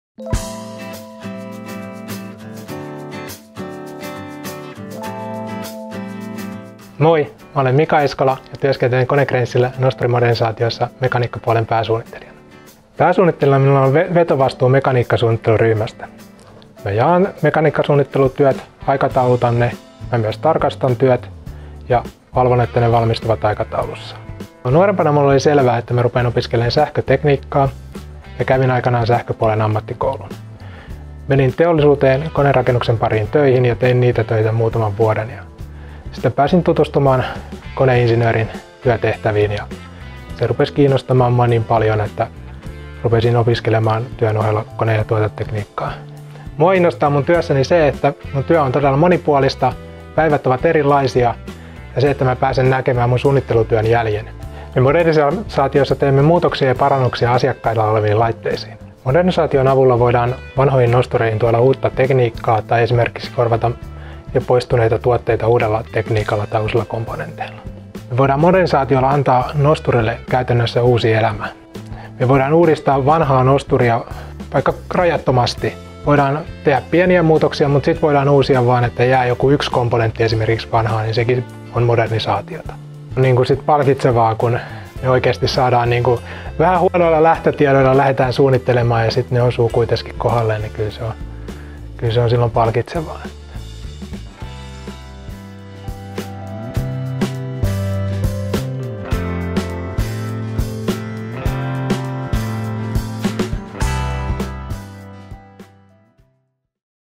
Career Story: Mika Eskola, Leading Engineer
“What I find inspiring in my job is that it is really diverse, every day is different, and I am able to see the results of my design work.”

Meet Mika Eskola, who works as a Leading Engineer in Service EMEA Finland Modernisation.